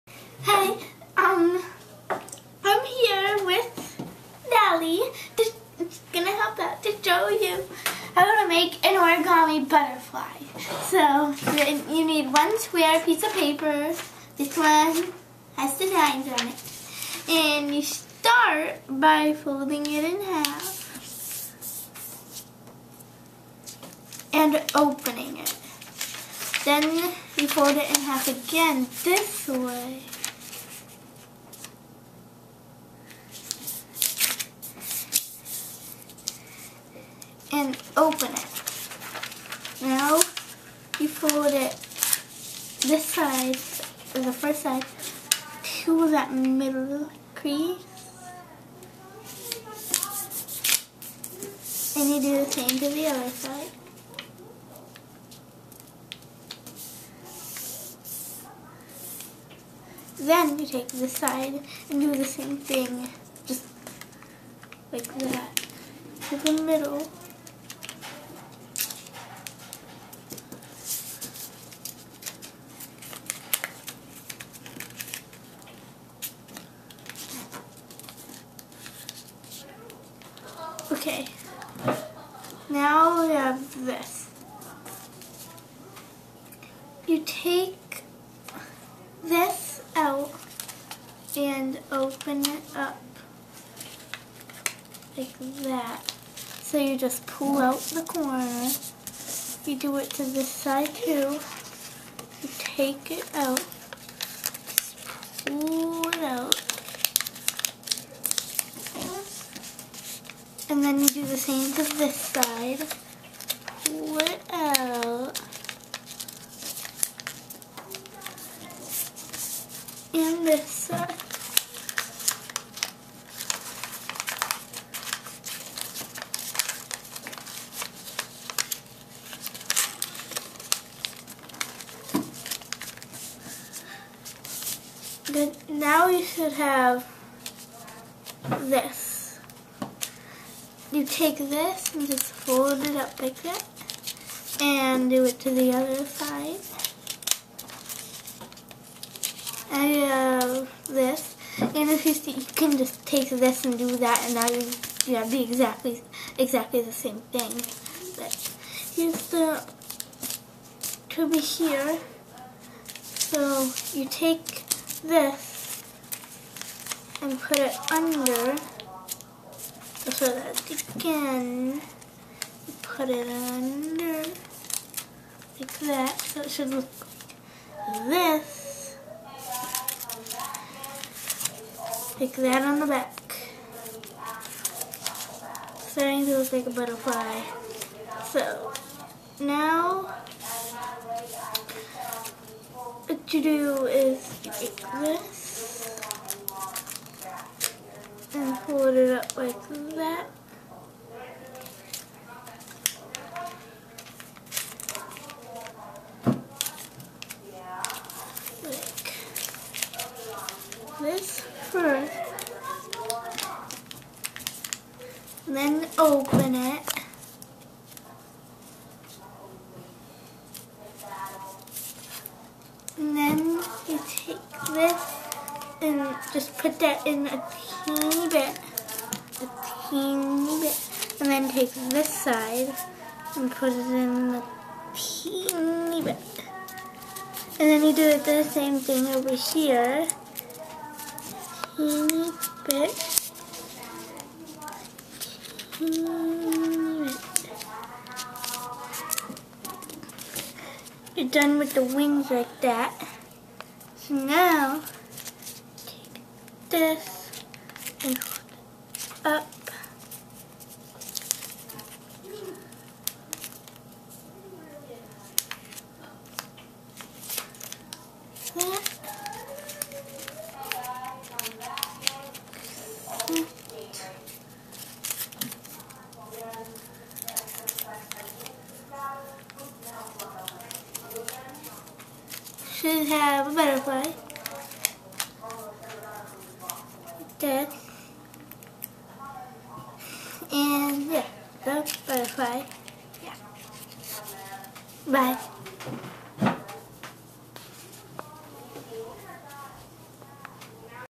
How To Make An Origami Butterfly - Easy Instructions
Heather demonstrates how to make an origami butterfly. She hopes you find these directions easy and fun to follow!